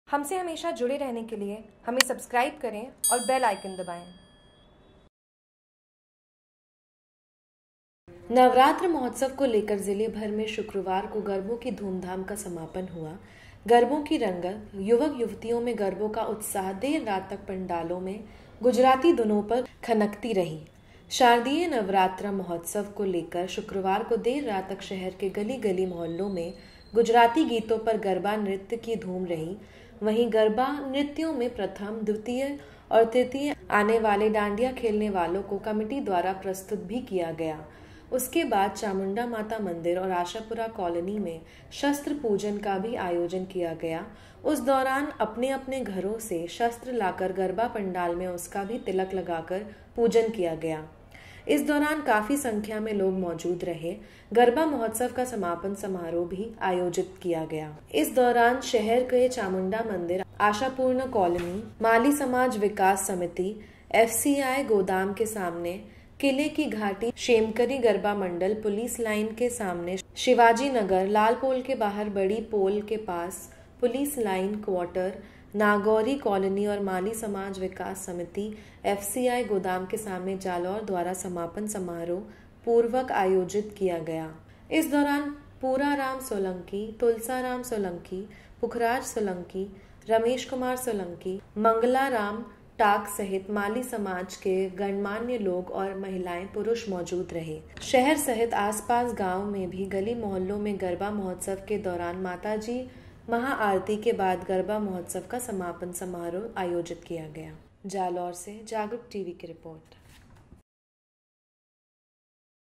JAGRUK TV : जालोर : आखरी दिन रही गारबो की धूम
जालौर। नवरात्र महोत्सव को लेकर जिलेभर में शुक्रवार को गरबो की घूम का समापन हुआ। गरबो की रंगत, युवक-युवतियों में गरबो का उत्साह देर रात तक पांडालो में गुजराती धुनों पर खनकती रही। शारदीय नवरात्रा महोत्सव को लेकर शुक्रवार को देर रात तक शहर के गली गली मोहल्लों में गुजराती गीतो पर गरबा नृत्य की धूम रही। वहीं गरबा नृत्यों में  प्रथम, द्वितीय व तृतीय आने वाले को डांडिया खेलने वालो को कमेटी द्वारा पुरस्कृत भी किया गया। उसके बाद चामुंडा माता मंदिर व आशापुरा कॉलोनी में शस्त्र पूजन का भी आयोजन किया गया। उस दौरान अपने अपने घरों से शस्त्र लाकर गरबा पंडाल में उसका भी तिलक लगाकर पूजन किया गया। इस दौरान काफी संख्या में लोग मौजूद रहे। गरबा महोत्सव का समापन समारोह भी आयोजित भी किया गया। इस दौरान शहर के चामुंडा माता मंदिर, आशापूर्णा कॉलोनी, माली समाज विकास समिति एफसीआई गोदाम के सामने, किले की घाटी क्षेमकरी गरबा मंडल, पुलिस लाइन के सामने, शिवाजी नगर, लाल पोल के बाहर, बड़ी पोल के पास, पुलिस लाइन क्वार्टर, नागौरी कॉलोनी व माली समाज विकास समिति एफसीआई गोदाम के सामने जालौर द्वारा समापन समारोह समारोह पूर्वक आयोजित किया गया। इस दौरान पूराराम सोलंकी, तुलसाराम सोलंकी, पुखराज सोलंकी, रमेश कुमार सोलंकी, रमेश कुमार सुंदेशा, भंवरलाल सांखला, टीकमाराम सांखला व  मंगलाराम टॉक सहित माली  समाज के गणमान्य लोग वह महिलाएं पुरुष मौजूद रहे। शहर सहित आस पास गाँवो में भी गली मोहल्लों में गरबा महोत्सव के दौरान माता जी महाआरती के बाद गरबा महोत्सव का समापन समारोह आयोजीत हुआ।